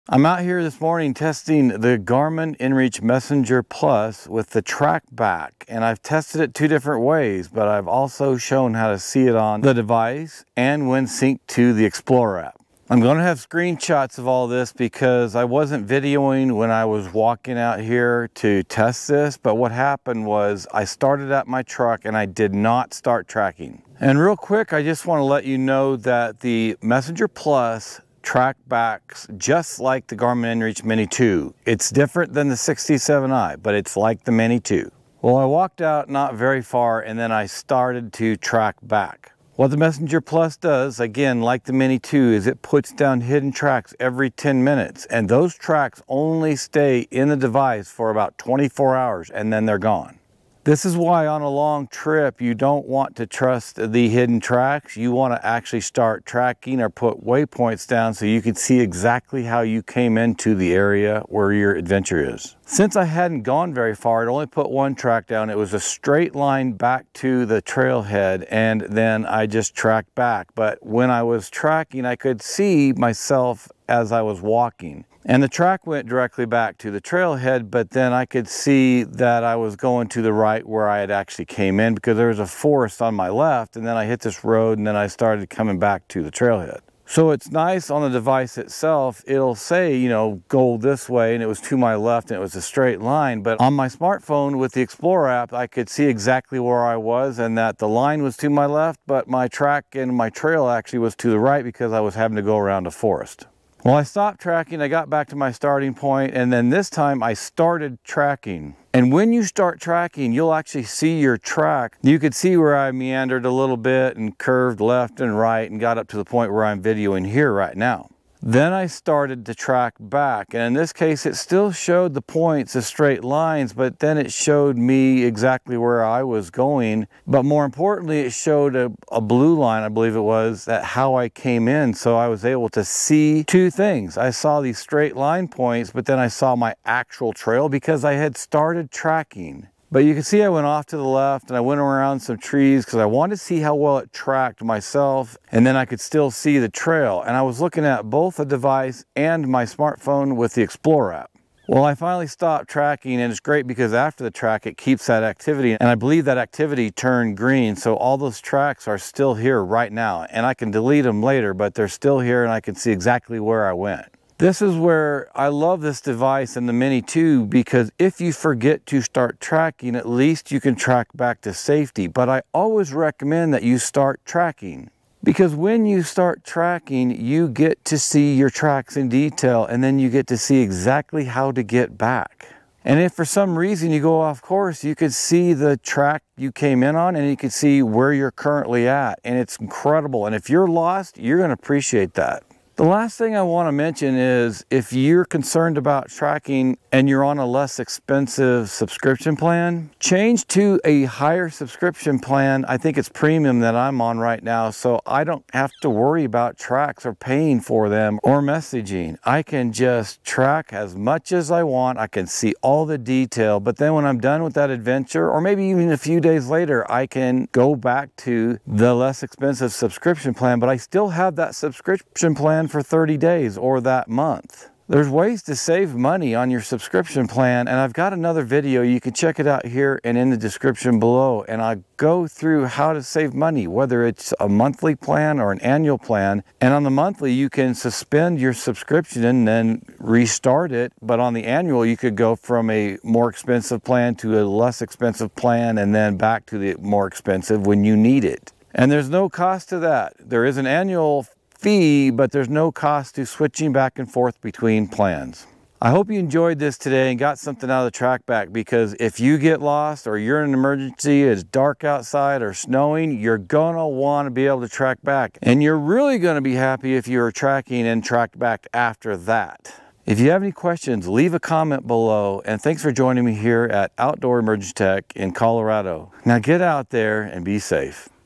TracBack with the Garmin inReach Messenger Plus
Learn how to TracBack with the Garmin inReach Messenger Plus.

✅ HELPFUL LINKS

✅ TIME CODES

00:00 Intro
00:20 TracBack without Tracking
01:46 TracBack when Tracking
03:12 Save on Your Subscription
04:15 Final Thoughts

Amazon
⚡️Garmin inReach Messenger Plus
⚡️Garmin inReach Messenger